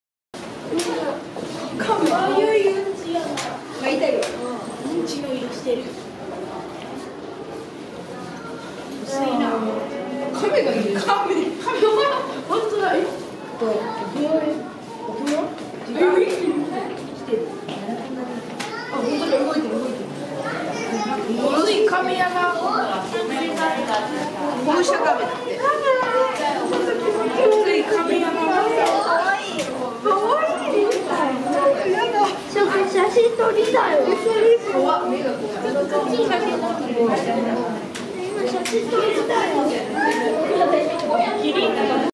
【猿の仲間】クロキツネザルとホウシャガメ（上野動物園 Ueno Zoo Ｈ30.2.9）
クロキツネザルとホウシャガメを撮影しました。